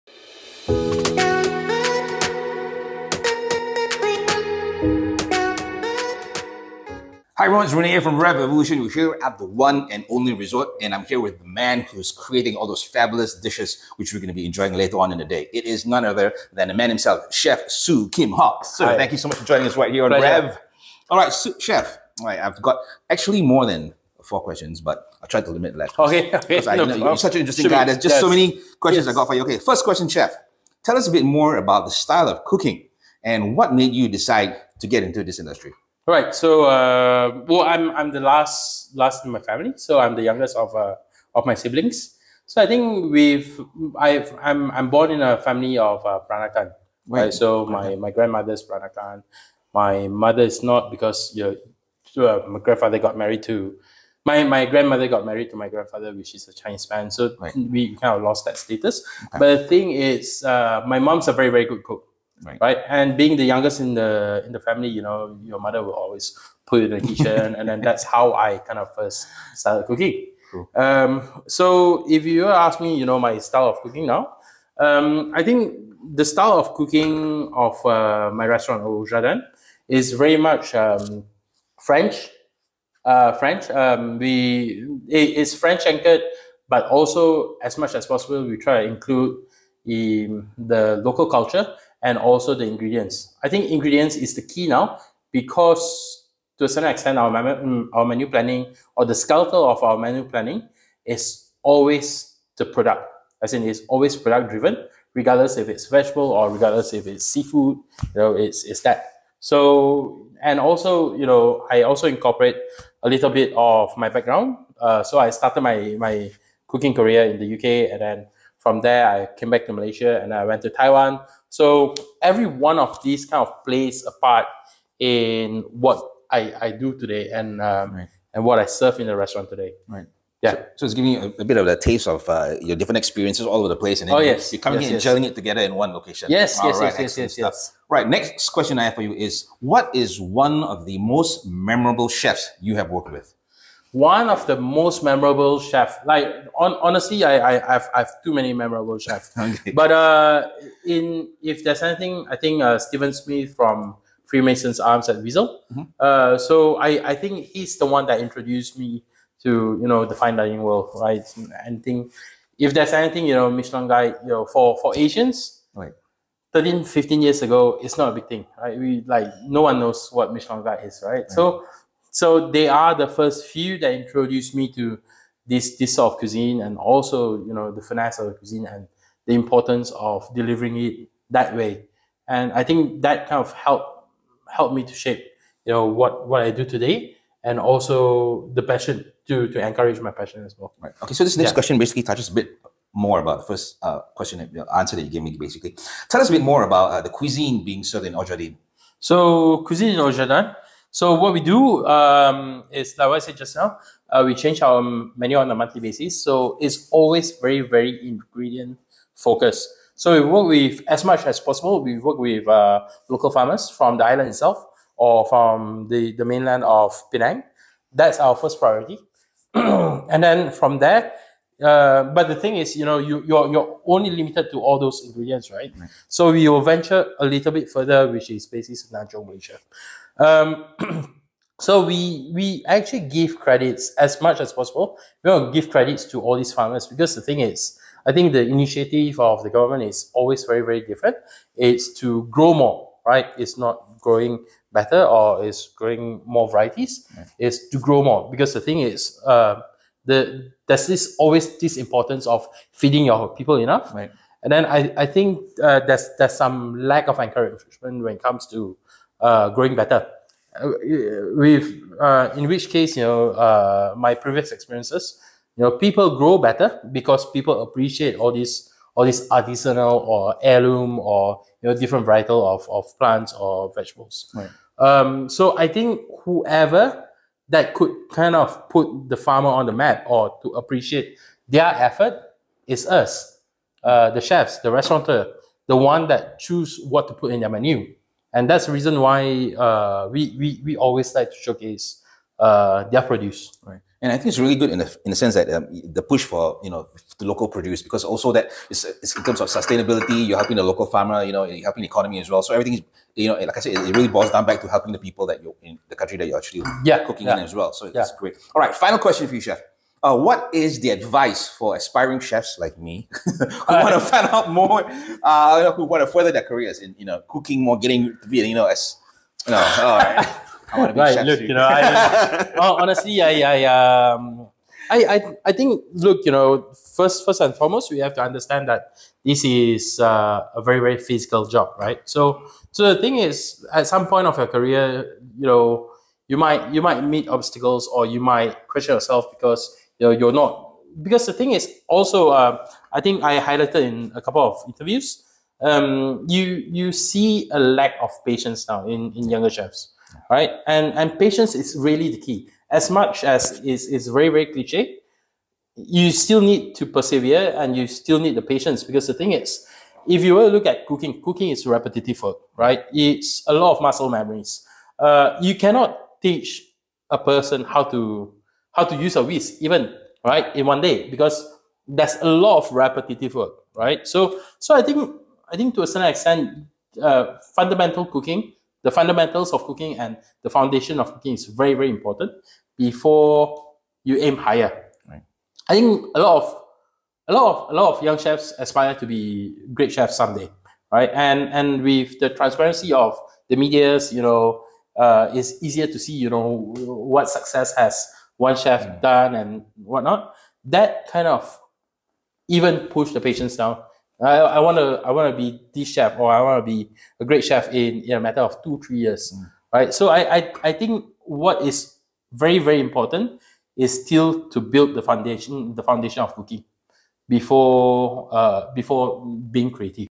The Michelin Chef Who Brought Back Korean BBQ
Revv Evolution dives deep into what makes Au Jardin Restaurant special, and uncovers some of the lesser known secrets of its stellar chefs. Chef Su Kim Hock of Au Jardin shares his unique take on Malaysian food, and how it's evolved over the years. Revv Evolution audiences get to hear about the restaurant's hidden gems and how you can experience them for yourself. Be sure to check out Chef Su Kim Hock's stellar restaurants Au Jardin and The.Reef.